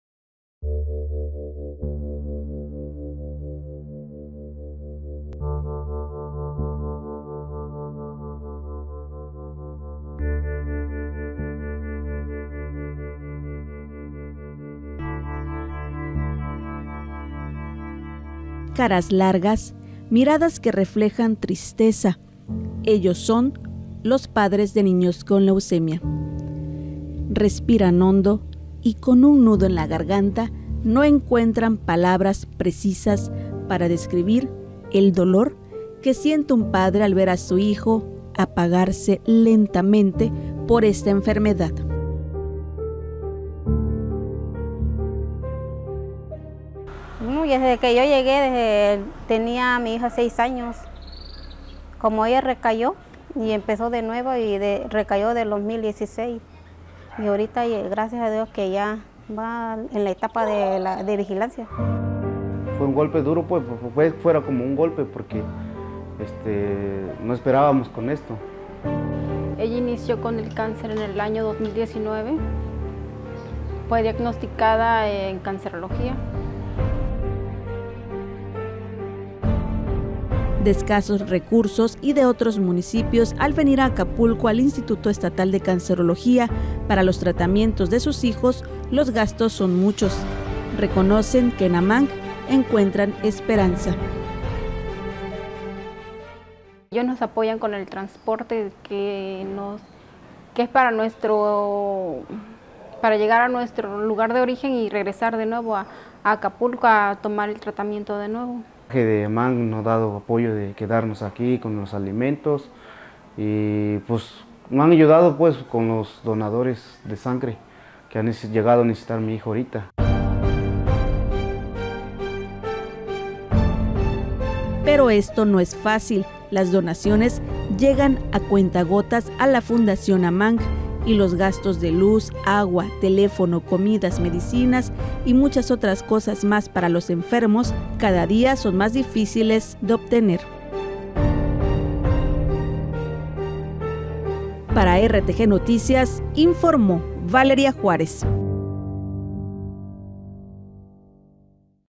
Reportaje 🎥 | El dolor de un padre de paciente con leucemia.
Reportaje 🎥 | Con un nudo en la garganta y una mirada que refleja tristeza, padres de pacientes con leucemia tienen que enfrentar las adversidades para que sus pequeños reciban el tratamiento y mejoren su condición.

A través de la Asociación Mexicana de Ayuda a Niños con Cáncer (AMANC) han encontrado apoyo para sobrellevar el proceso con alimento y un lugar seguro para pasar la noche.